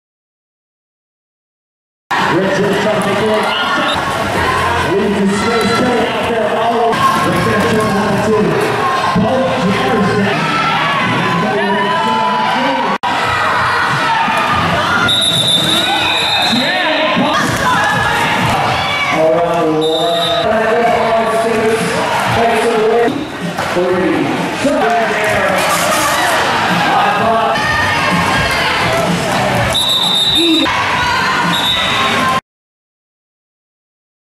hits and falls of Toxic and Atomic bout
Cast Iron Skaters sponsored a bout between Toxic 253 and Atomic City Sept 28 2013! Here are a few clips of the biggest hits and falls of the evening.